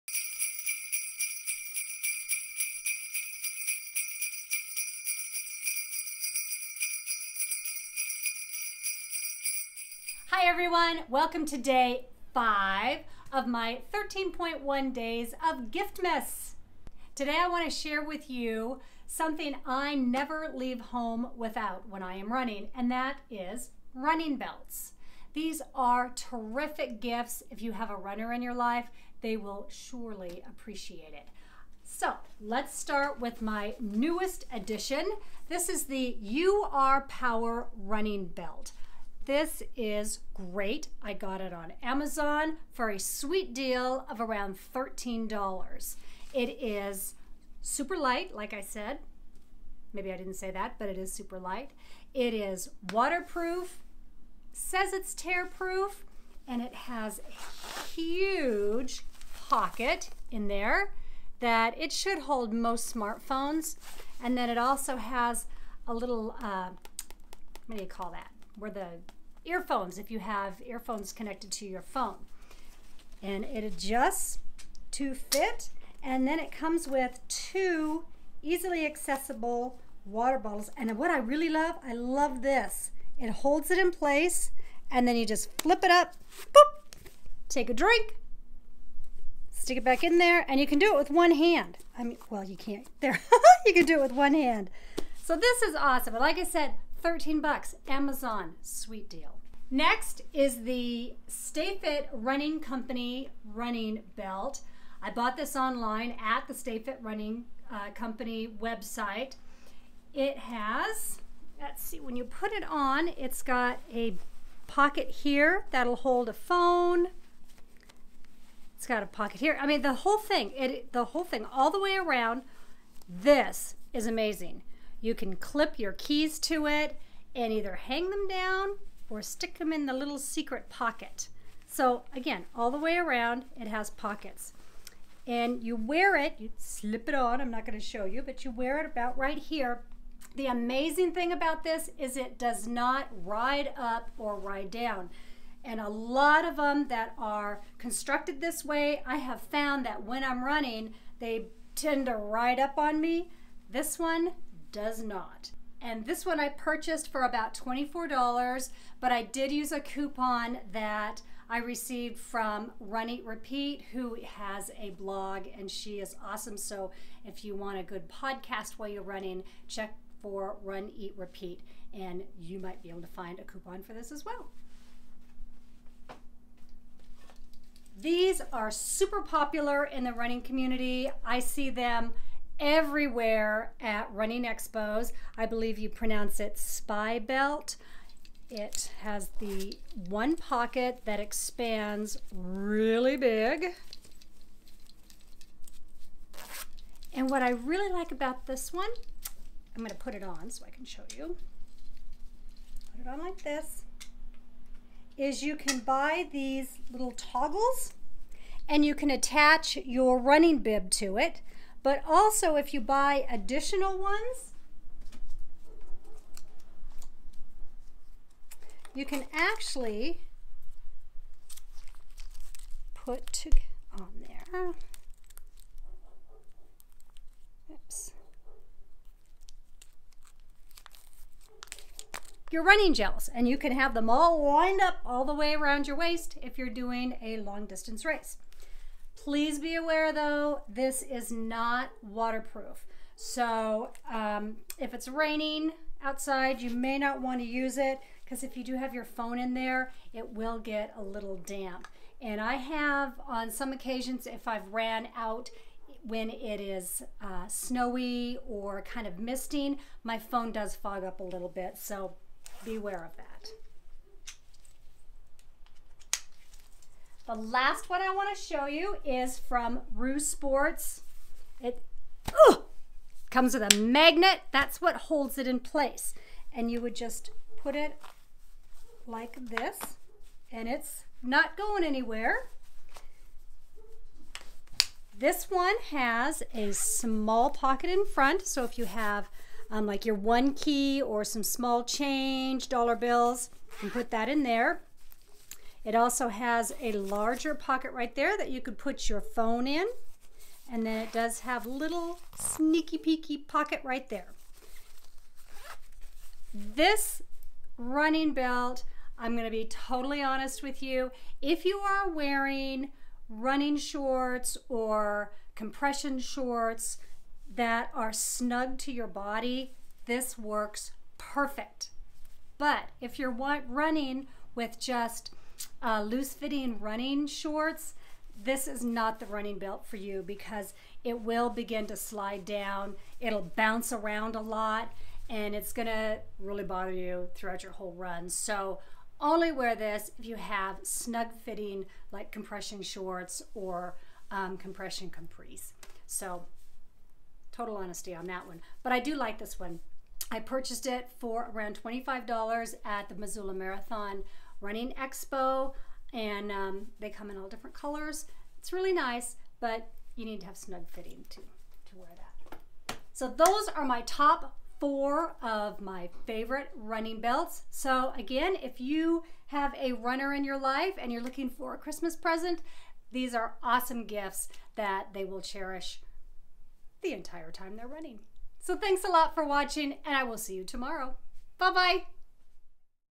Gift-Mas Day 5 (Favorite Running Belts)
I love running belts. Here are a few I can’t live without. Share your or favorites in the comments below.

To order

URpower running belt

Roosport running belt